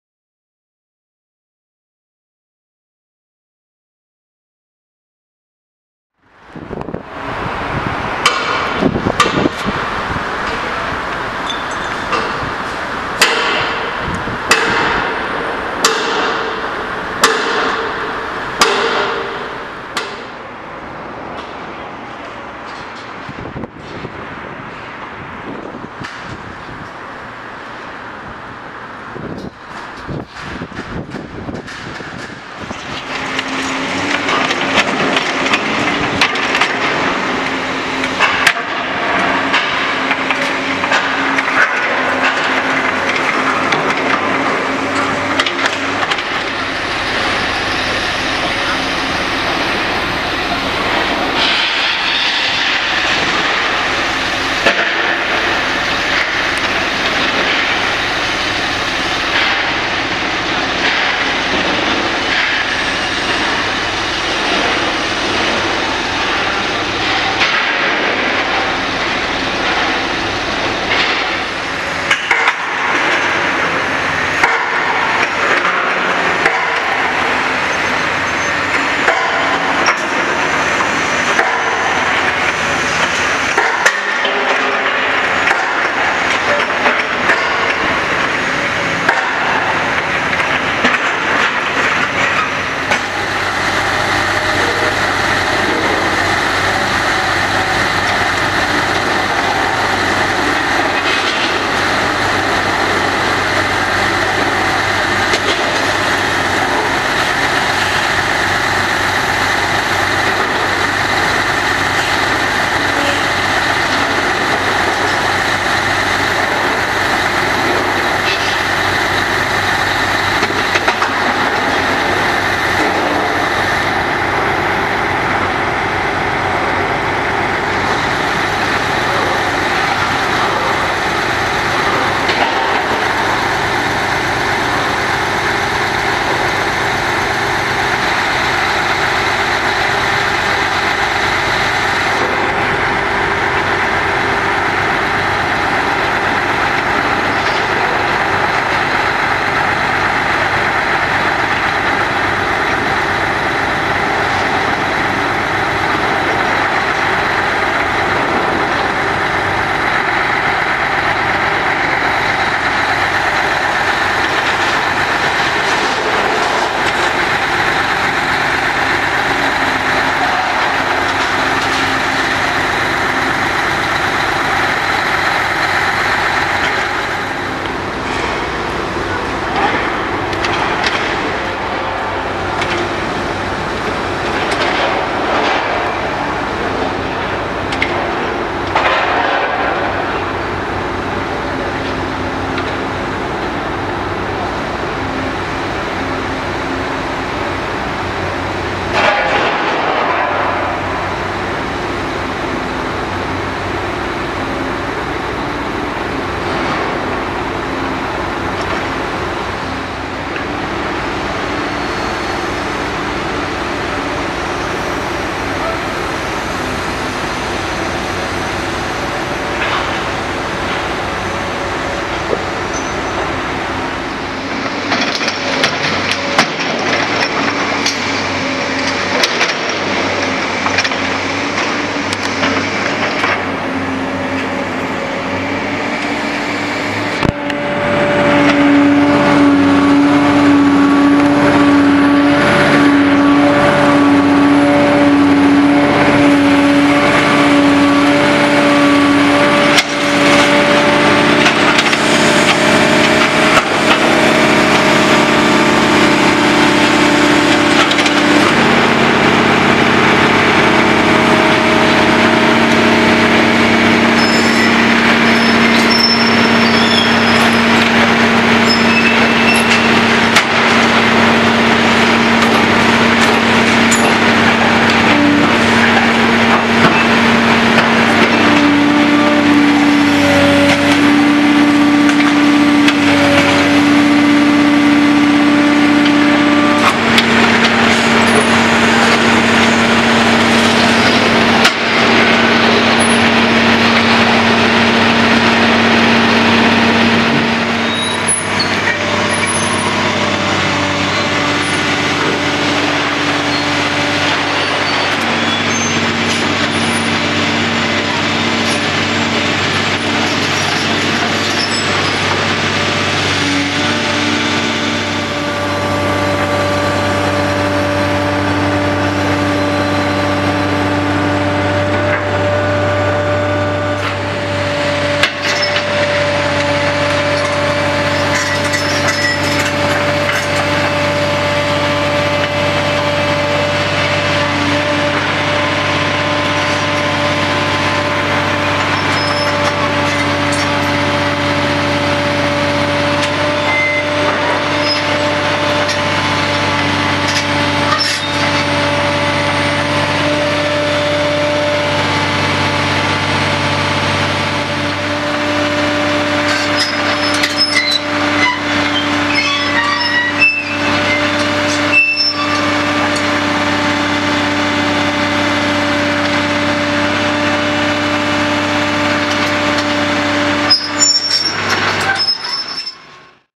CATERPILLAR 385C LONGFRONT BAGGER AUFBAU # ABBRUCH BODO FREIMUTH
▀▄▀▄▀▄  CLICK FOR MORE  ▼▼▼  MEHR INFOS  ▄▀▄▀▄▀

Abriss des Sachsenburg Bürogebäudes in Hamburg: das riesige Bauwerk muss Platz für einen modernen Bürokomplex machen.

🔔 Aktiviere die GLOCKE beim Abo-Button direkt unter dem Video für aktuelle Benachrichtigungen! 🔔

Dir gefallen meine Videos?

●▬▬▬▬▬▬๑۩۩๑▬▬▬▬▬▬●

Tearing down huge office building in Hamburg, Germany: it will be replaced with a modern building.

You like my videos?